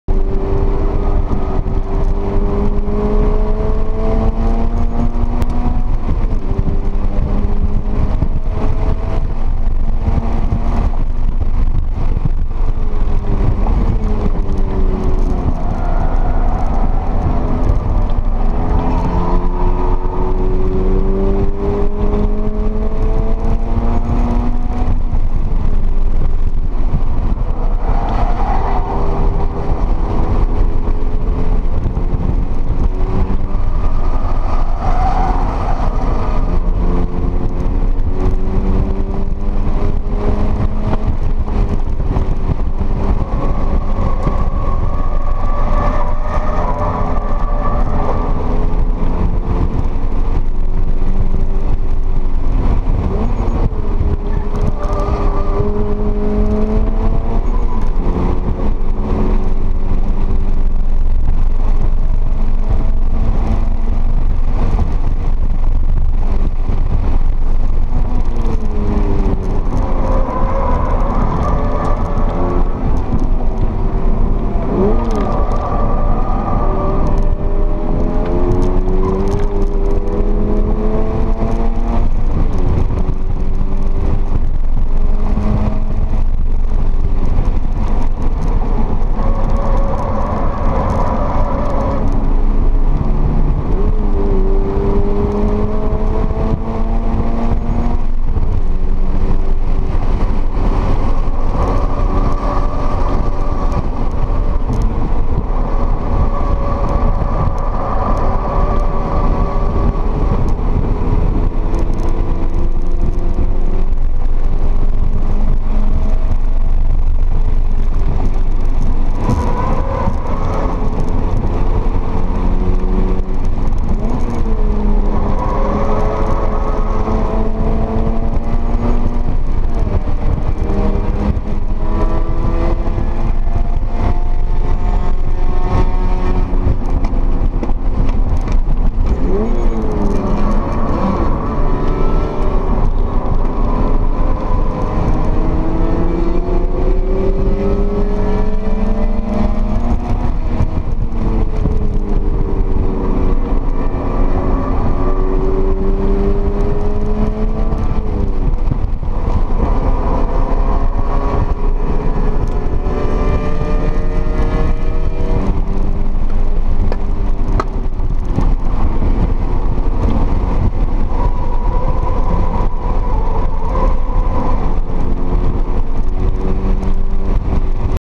20220320 Chuckwalla CVR CCW GR86 V730 2:03.4
Stock 2022 Toyota GR86
2:03.4 lap
Stock except for :
PowerStop Trackday Spec pads
ProjectMu brake fluid
Camber bolts
225-40/17 Kumho V730
17 x 8 et45 rims